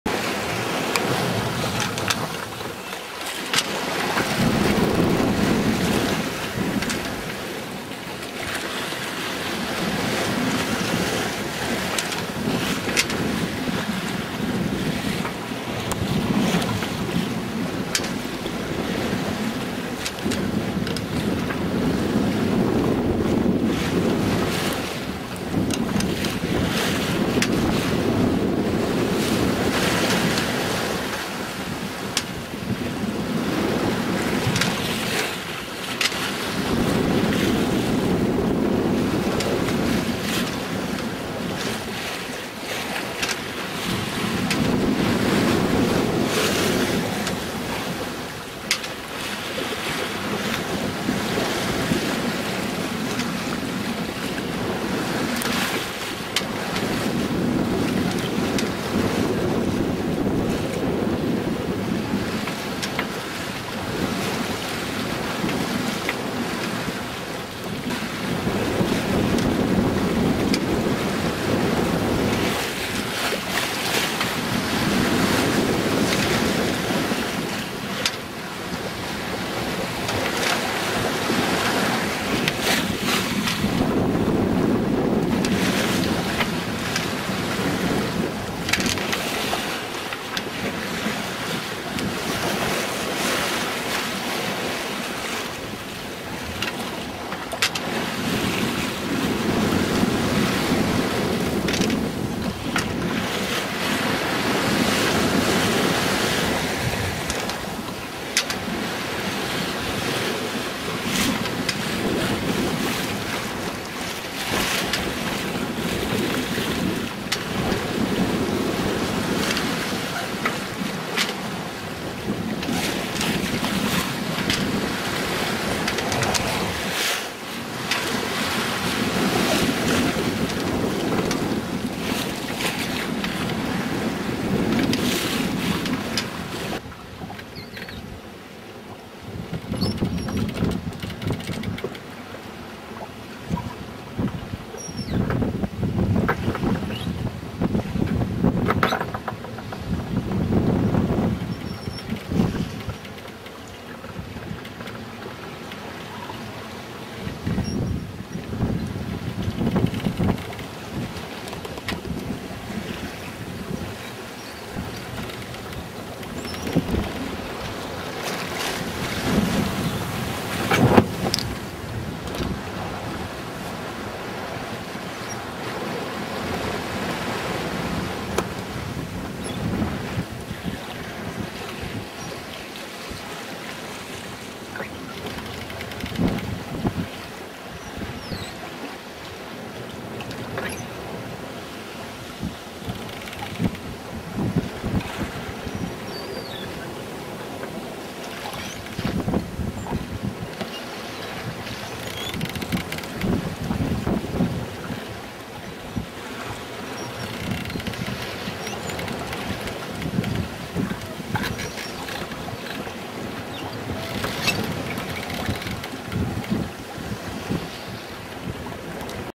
CourseStabilityBenchmark_2
The "WindGear by Joern Heinrich" self steering device proofs a very good course stability on the broad reach. This is due to a unique and brand new development: Yaw Dampening Gear (YDG). Most servo pendulum wind vanes sail very stable from beam reach to close hauled, but lack course stability on wavy broad reach to running point of sail. The YDG is a mecanics added to the more or less traditional servo pendulum oar principle and cures the problem.
conditions: LoA: 7,6 m, Wind: 10m/s, 150°, Sea state 0.5-1m